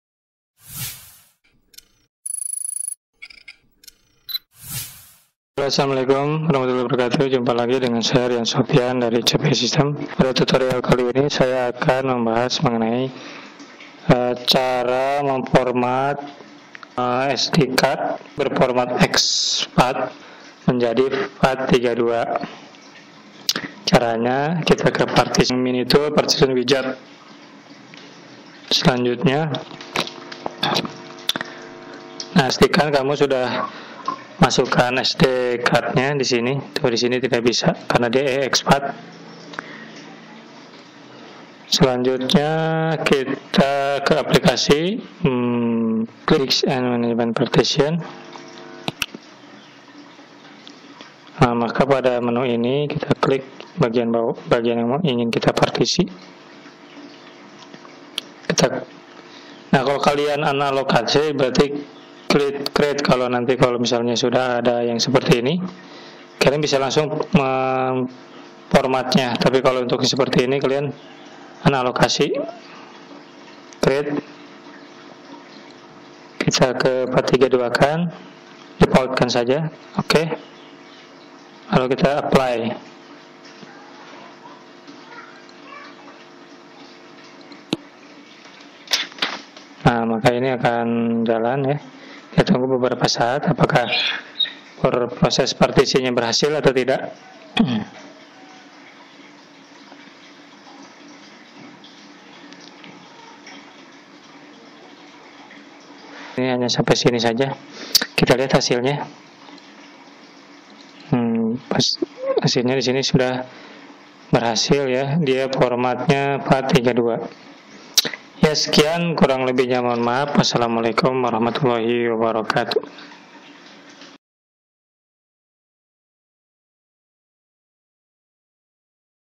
Cara Format dari EXFAT ke FAT32
FD atau microsd baru teman-teman teryata tidak terbaca ? jangan panik dulu gan mudah ko caranya yaitu convert saja formatnya dari exfat ke fat32 silahkan ditonton videonya jangan lupa subscribe dan comentnya ya :)